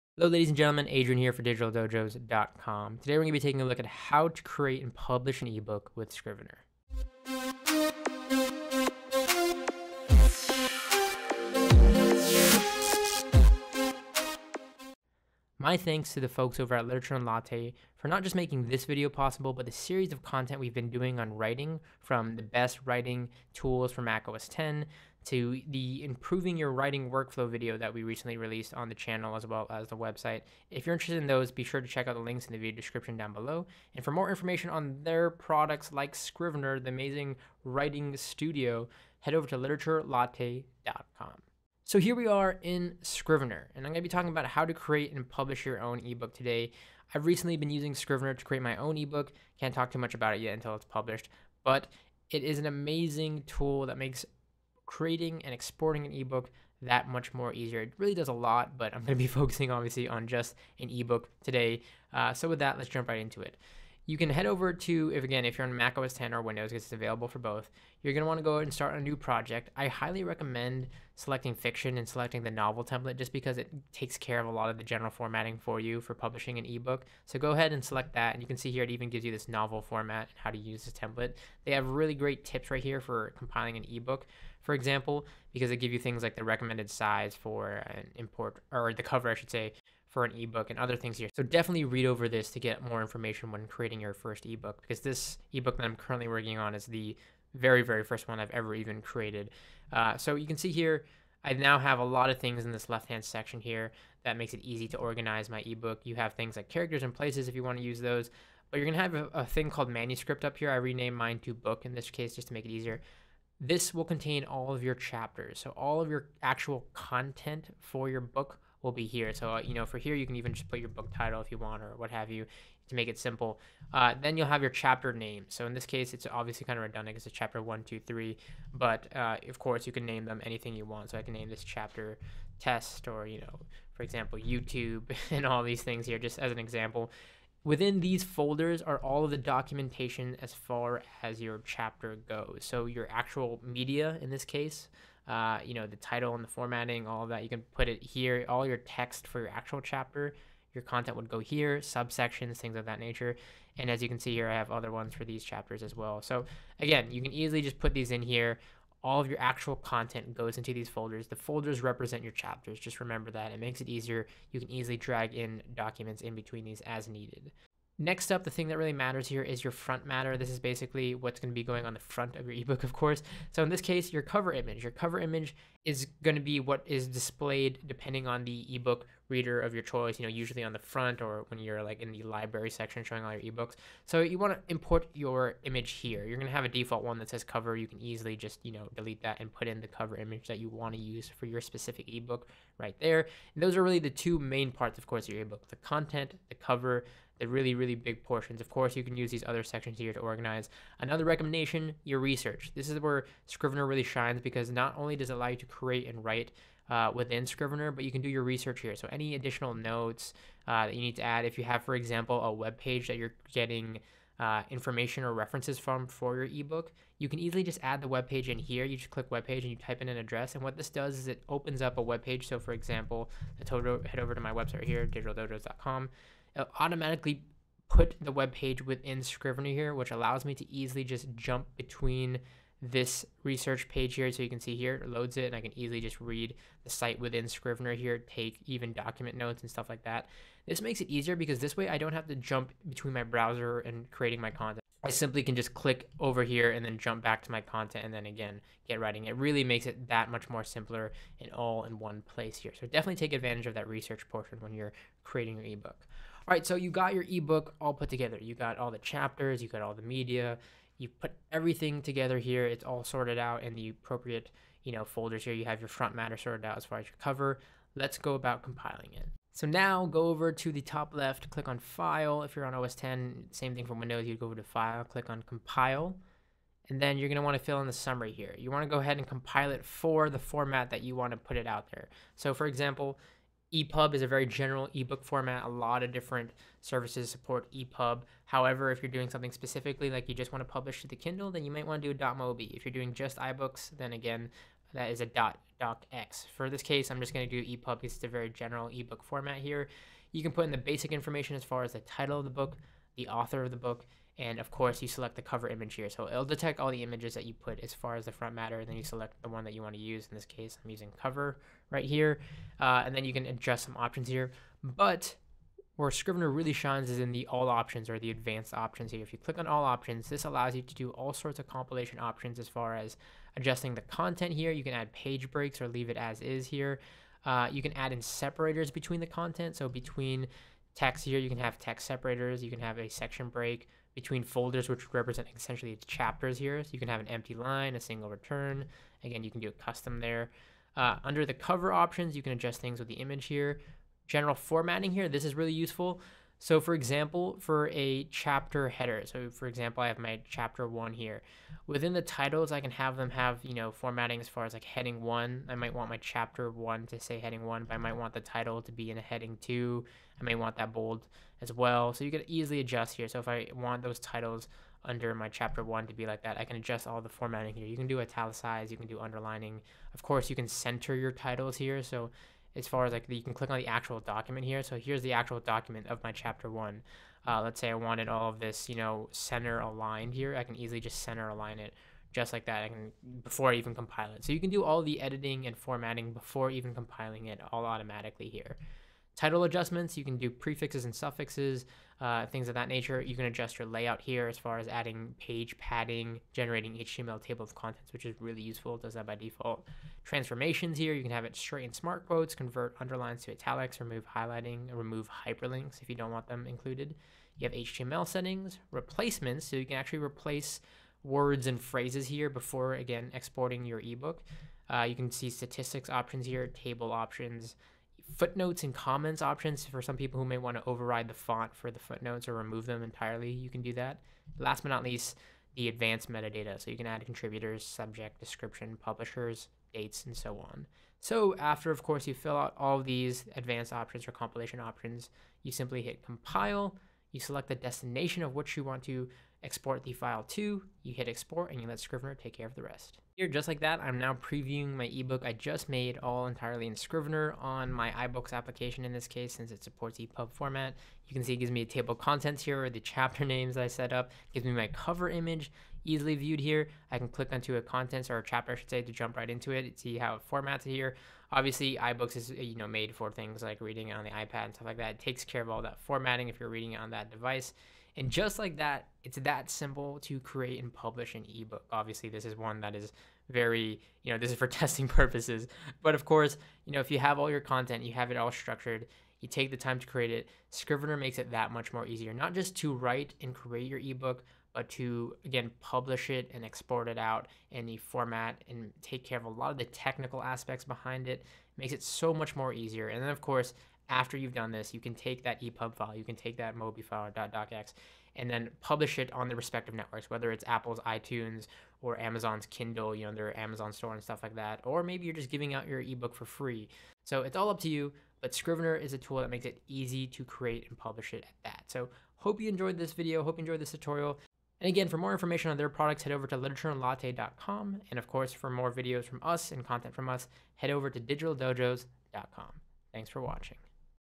How To: Create & Publish an eBook w/Scrivener
A tutorial on creating & publishing an eBook using Scrivener.